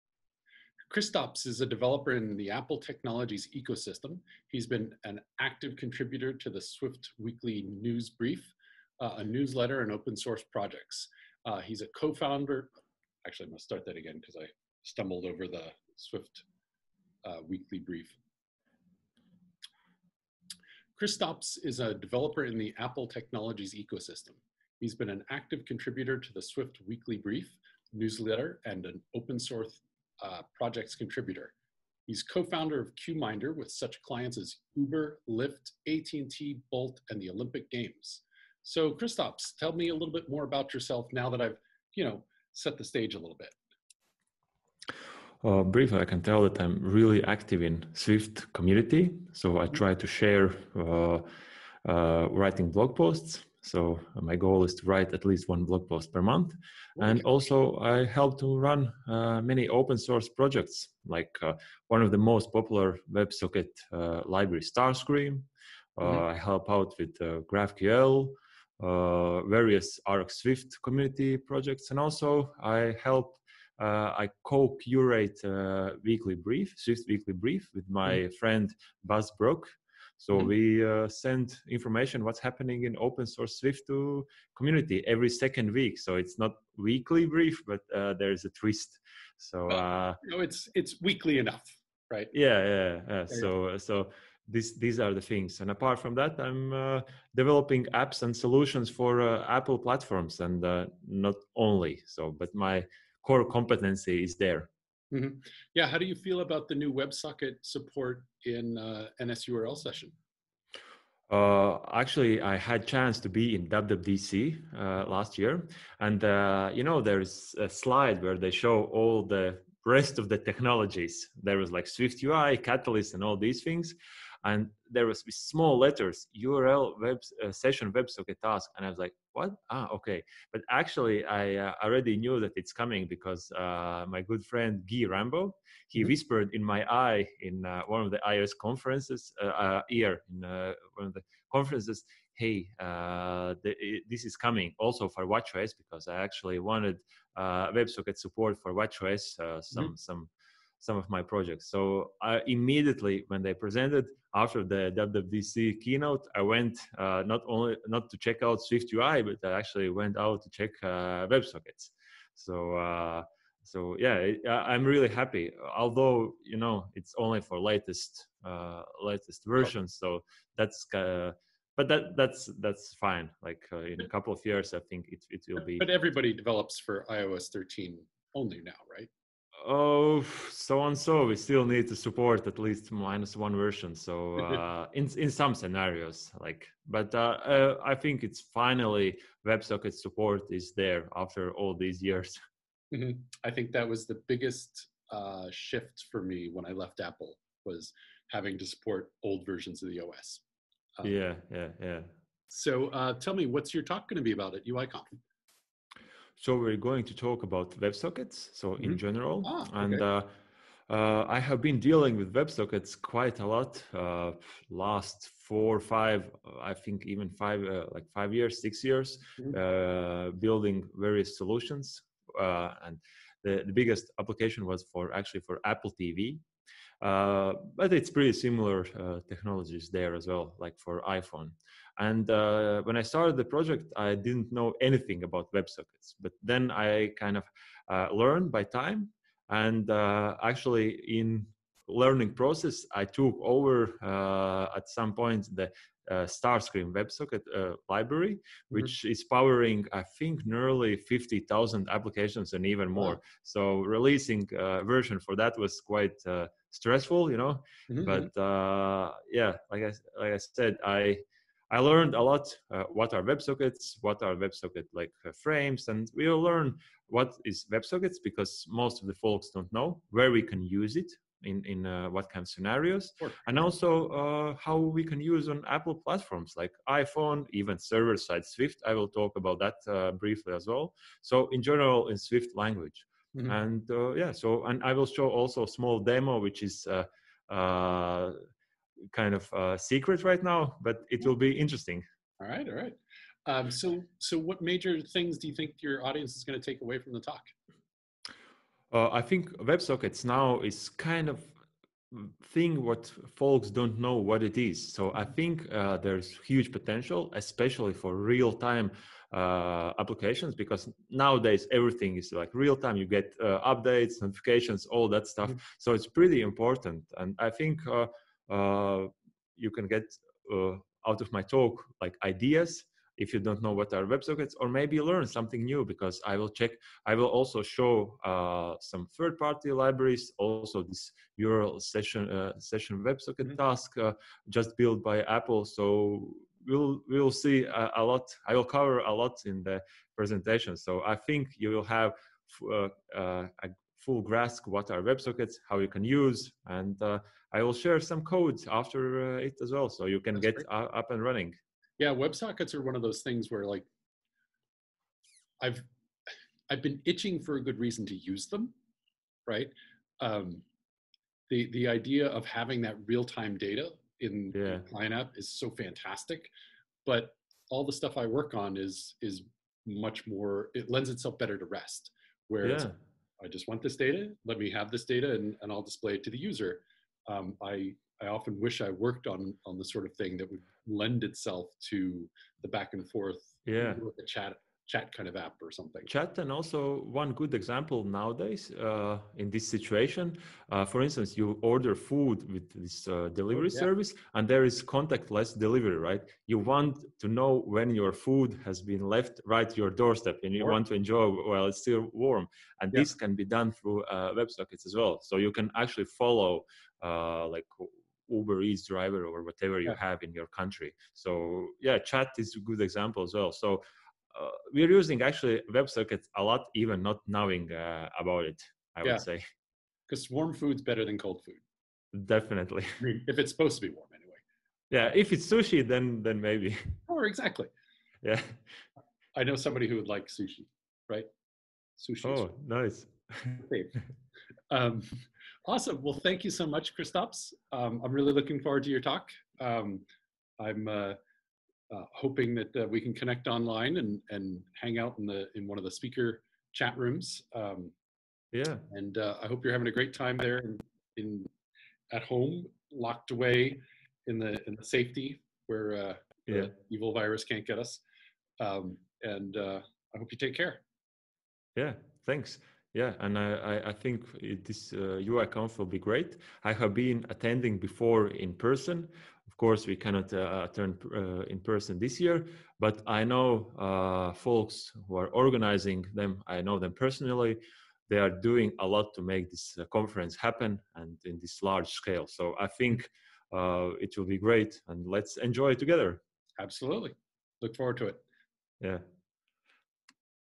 UIKonf 2020 - A short interview with Kristaps Grinbergs
Kristaps Grinbergs is an Apple technologies software engineer. Active contributor at Swift Weekly Brief newsletter and open-source projects. Executed as a co-founder at a global-scale company Qminder that serviced clients such as Uber, Lyft, At&T, Bolt, The Olympic Games and more.”

**His talk: We need to talk about Websockets**

If you're tired of slow and flaky networking, this talk will explain to you the state-of-the-art solution: Websockets. Using technologies such as Apple's freshly announced URLSession Websockets, incredible new open source SwiftNIO and Network.framework, we'll look at how you can open a two-way socket between your app and server to accelerate your networking, allowing you to communicate without disruptions, do so securely, and even learn some tips and tricks from Apple. The speaker is the maintainer of Starscream, one of the most popular Swift Websocket libraries.